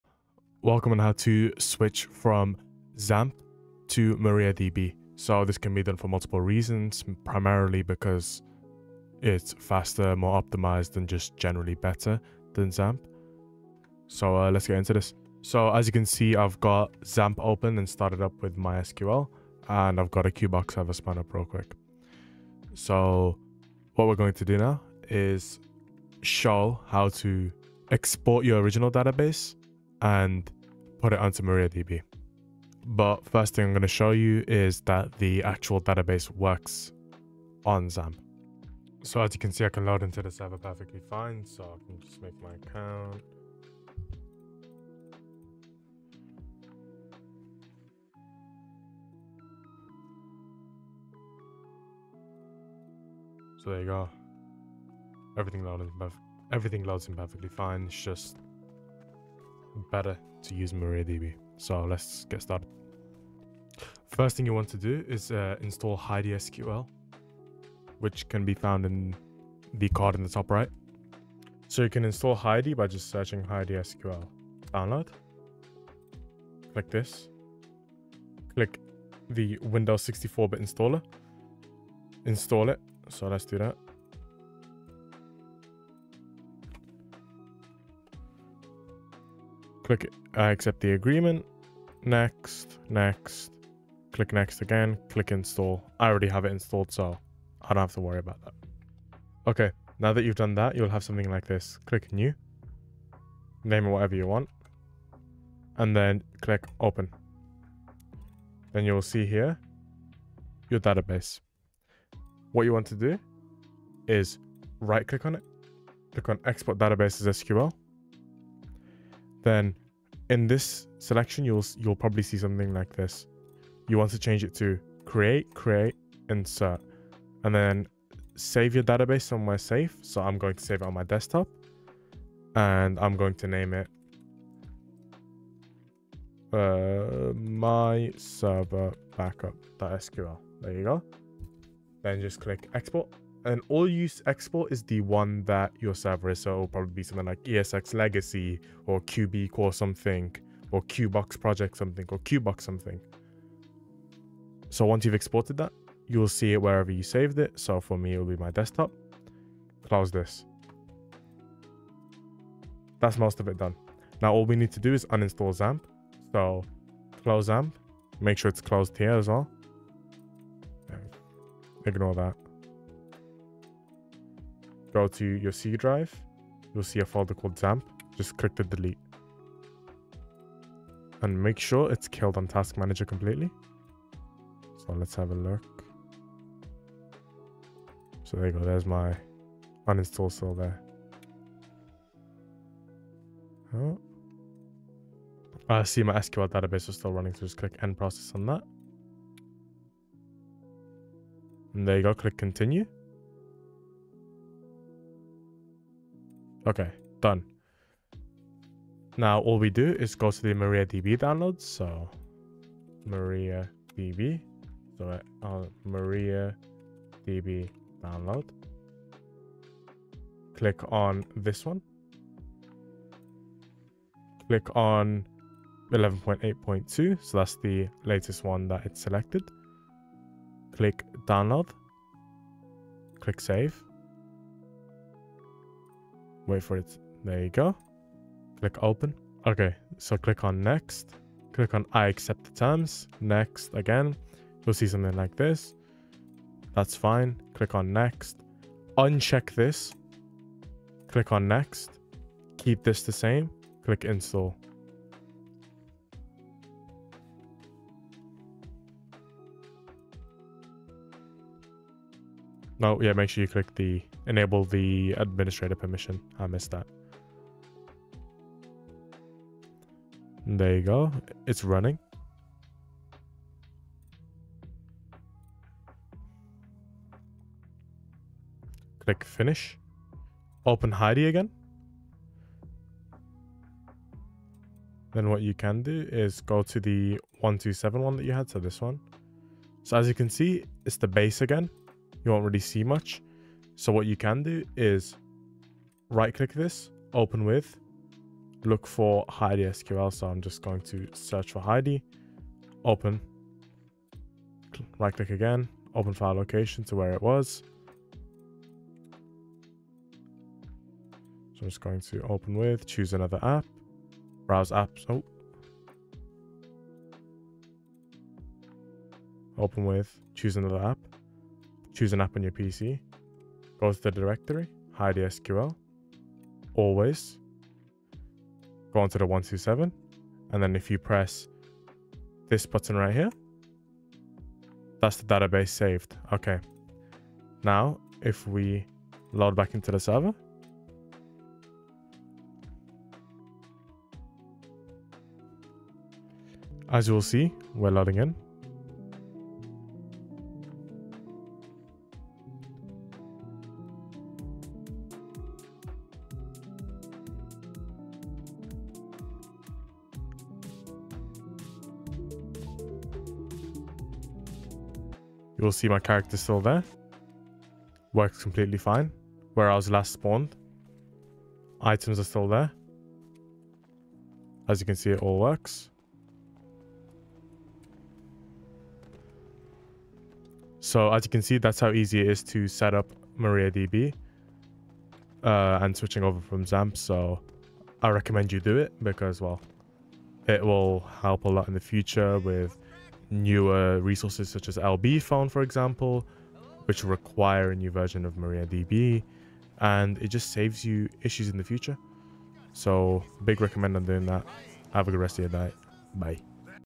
How to transfer from XAMPP to MariaDB in FiveM
TIMESTAMPS:

00:00:00 - Introduction
00:00:35 - Old XAMPP Database Working
00:01:05 - Install HeidiSQL
00:01:39 - Open HeidiSQL + Export Old Dabase
00:02:43 - Uninstall XAMPP
00:03:34 - Install MariaDB
00:04:40 - Import XAMPP Database into MariaDB
00:05:45 - It works :)